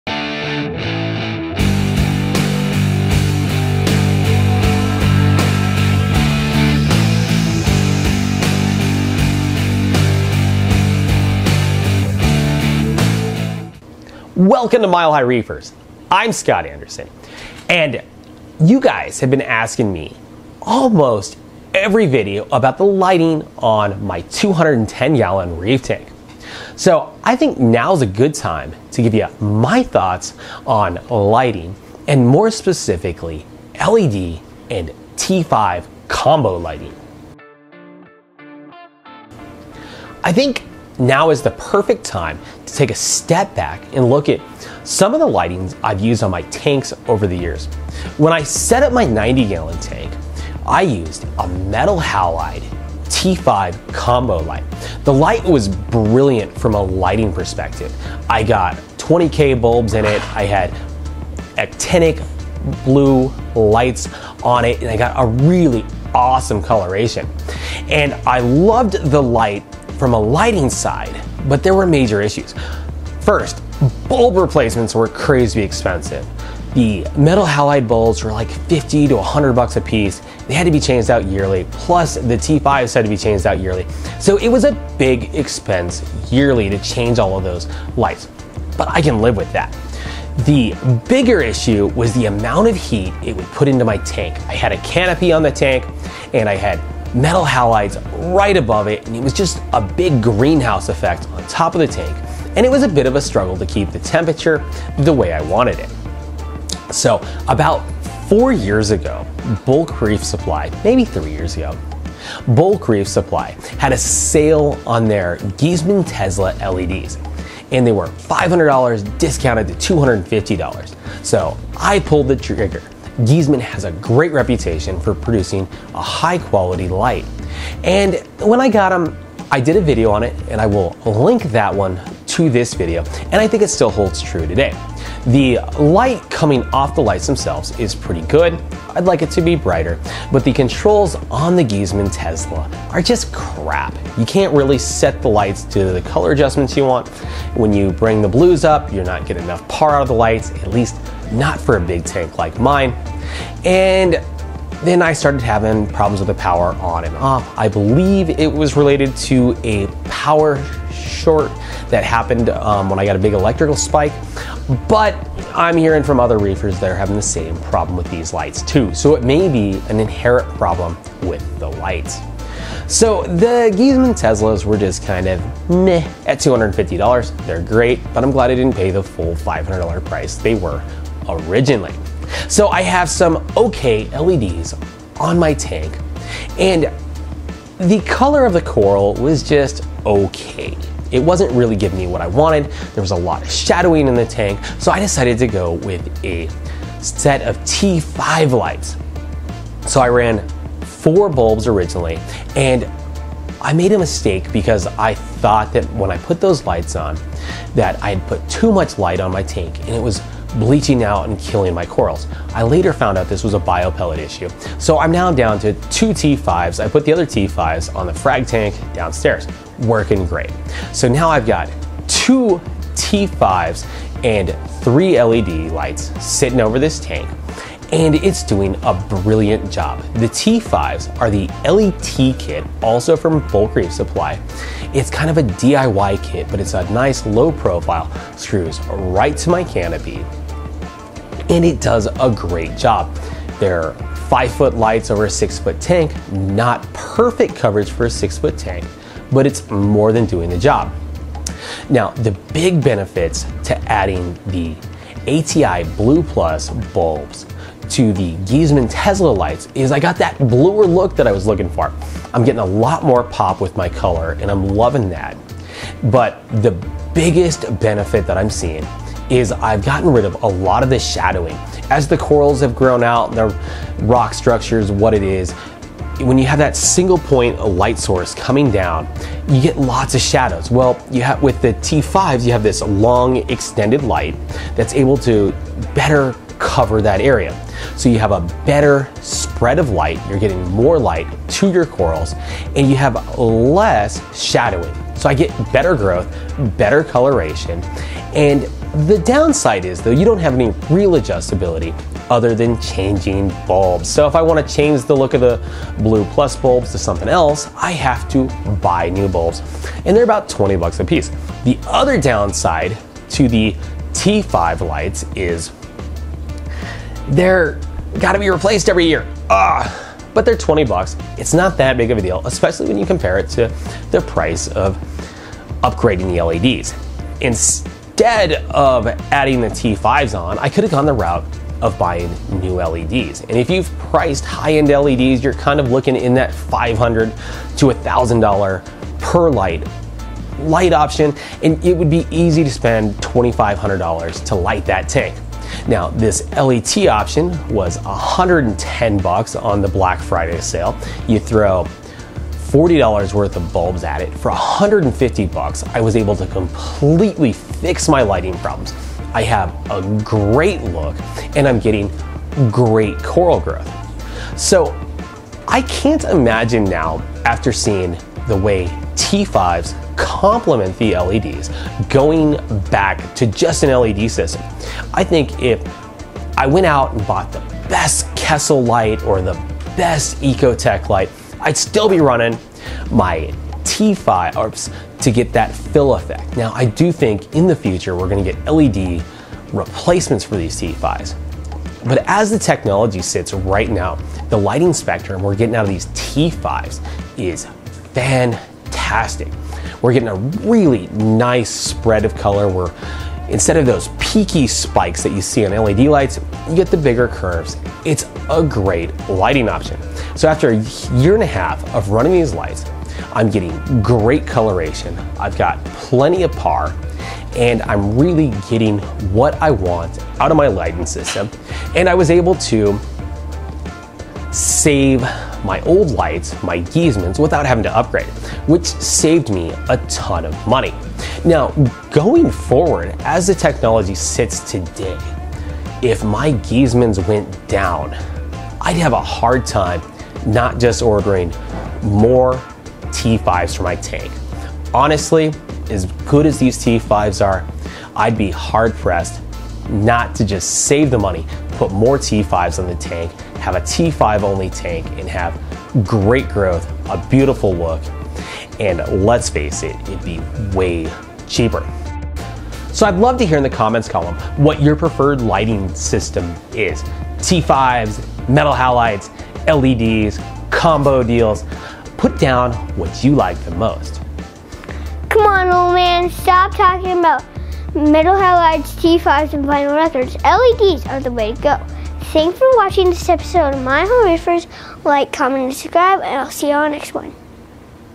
LED T5 Combo Lighting Long Term Review
In this episode I give my long term thoughts to running a T5 LED combo lighting system on my reef tank.

Giesemann Teszla LED Reef Tank Lights Full Review!